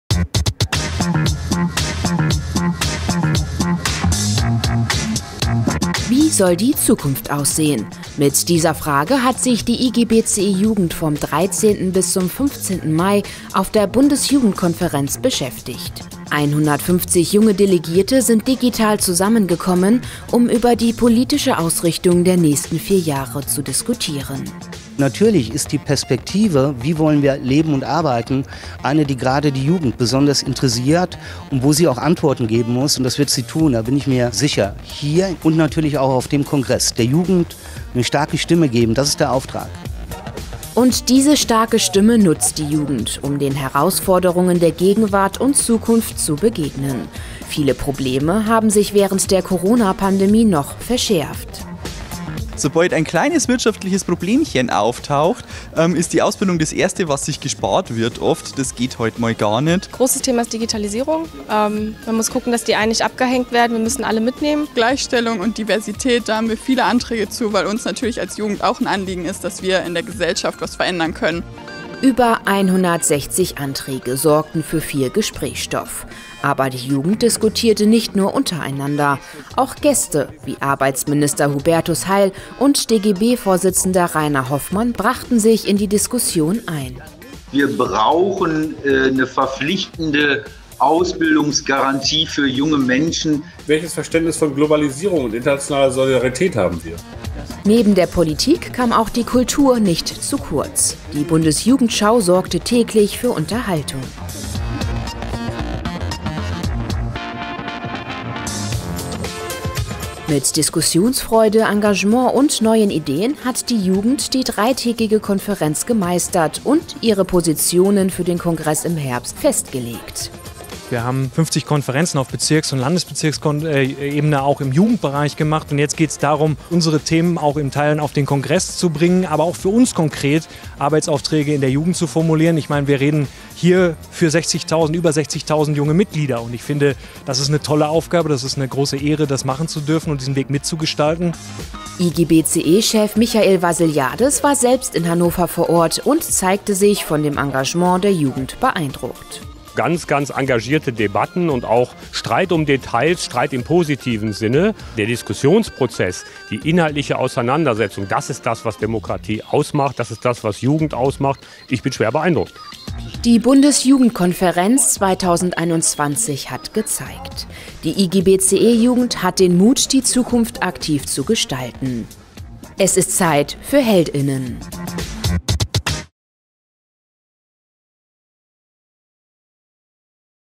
"Zeit für Held*innen"
Mehr als 100 junge Delegierte sprachen über die wichtigsten Themen der Zukunft. Neben der Teilnahme politischer Gäste und einem Kulturprogramm wurden auf der Bundesjugendkonferenz vom 13. bis 15. Mai vor allem zahlreiche Anträge diskutiert und beschlossen.